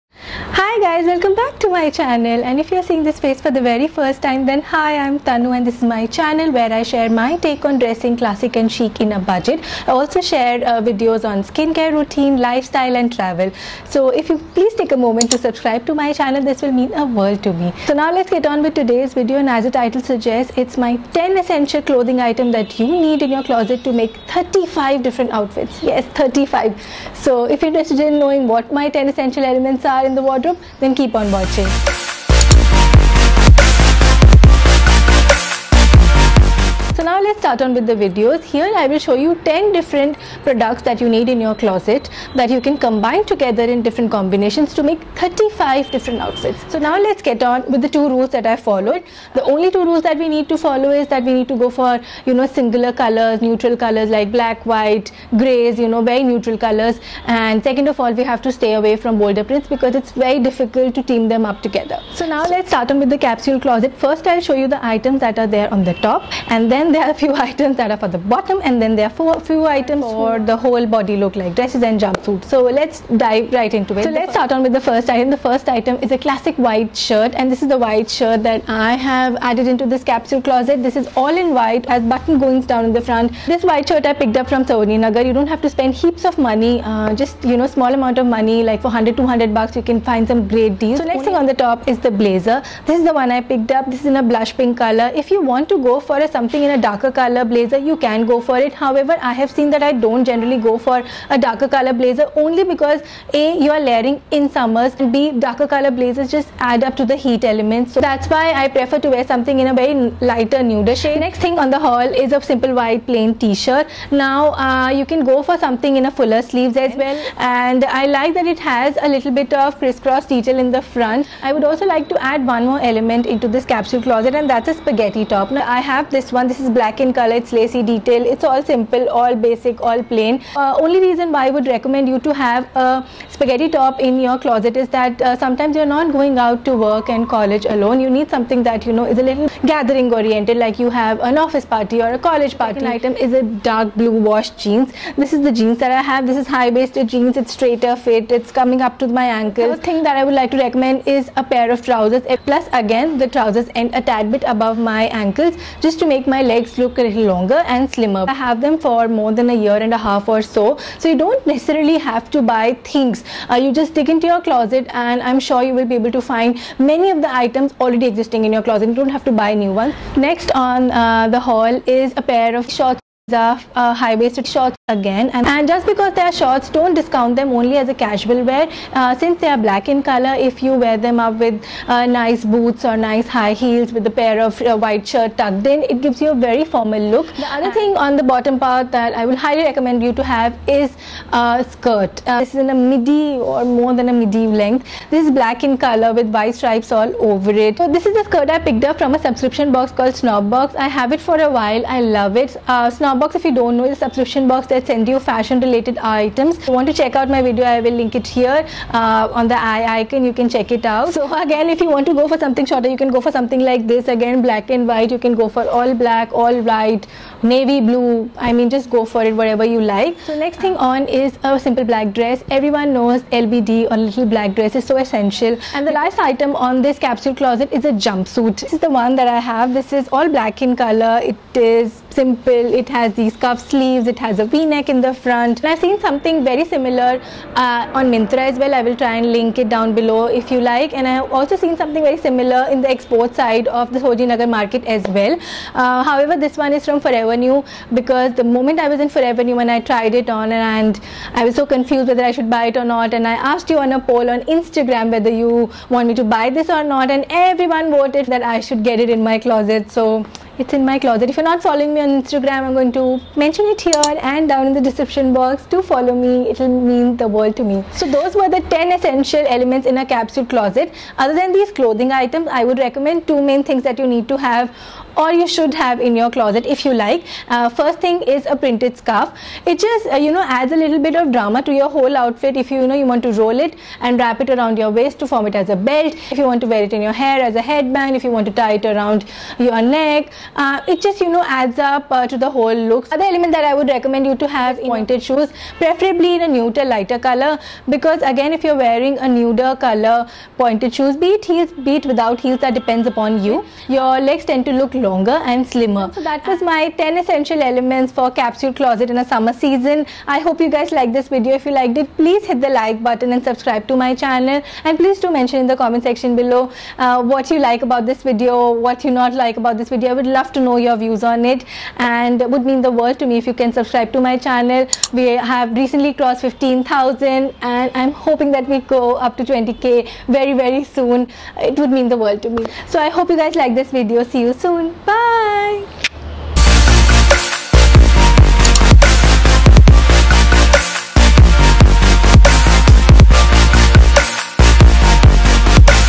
35 CUTE SUMMER OUTFITS | 10 ESSENTIAL CLOSET ITEMS| COLLEGE & OFFICE WEAR GUIDE | Tanu Gupta
Hey Cuties,
Since we all struggle to find the right outfit. Thats when I decided to solve this problem for my self and chose 10 essential items in the wardrobe that helps you make 35 different outfits.

the 10 essential items are AS FOLLOWING;
I will try and link similar items below as well .

xoxo
Tanu Gupta
Tanz Worldd